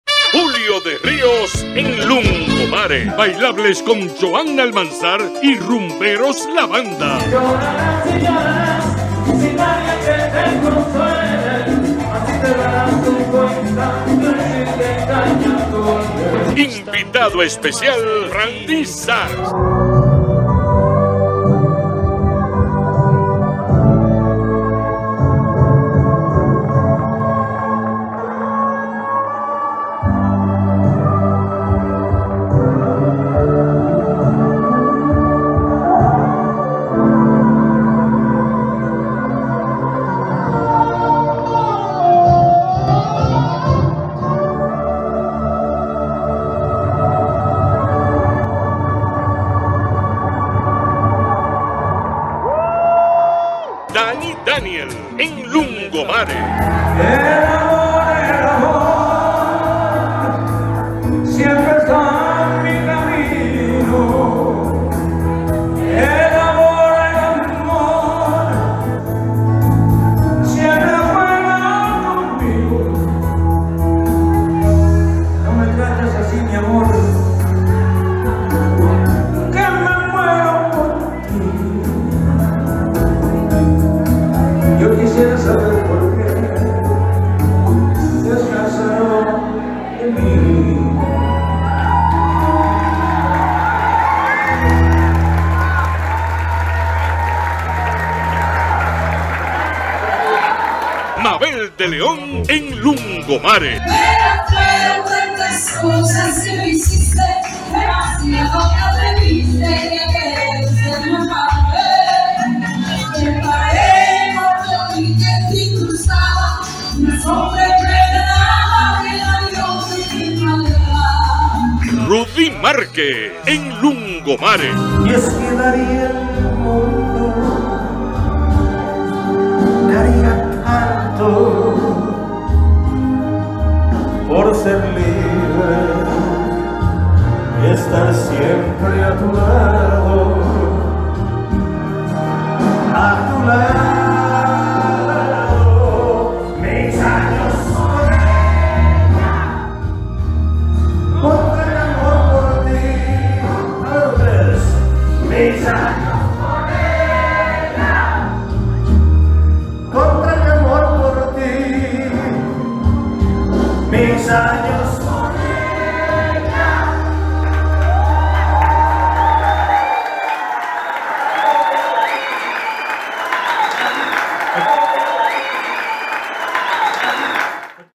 OCURRIO EN LUNGOMARE PARA PAPA
Julio De Ríos, la serie de conciertos en tributo a Anthony Ríos concluyo el viernes 26 de julio con Danny Daniel, Johanna Almanzar con Rumberos La Banda y Frandy Sax, mientras que el sábado 27 Rudy Márquez, Los Soneros y Mabel De León deleitaron a un amplio público en Lungomare del malecón.